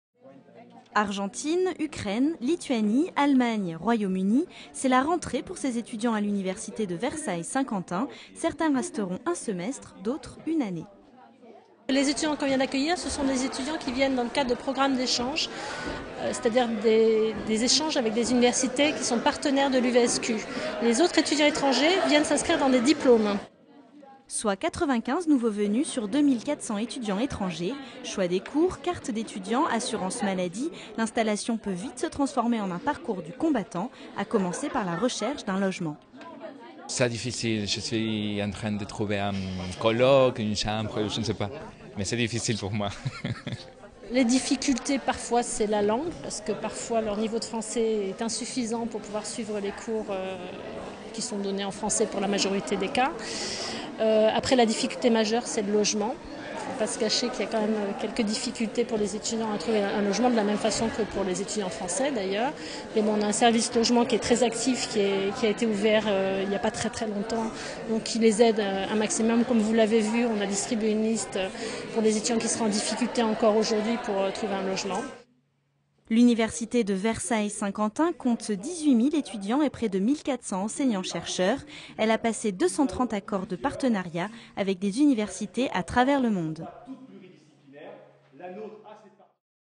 L'Actu - la rentrée des étudiants étrangers
Une rentrée particulière, ce lundi 10 septembre, à l'université de Versailles-Saint-Quentin, celle des étudiants étrangers. La volonté pour l'UVSQ d'ouvrir de plus en plus ses portes aux étudiants d'ailleurs. Une façon d'étendre sa popularité au-delà des frontières.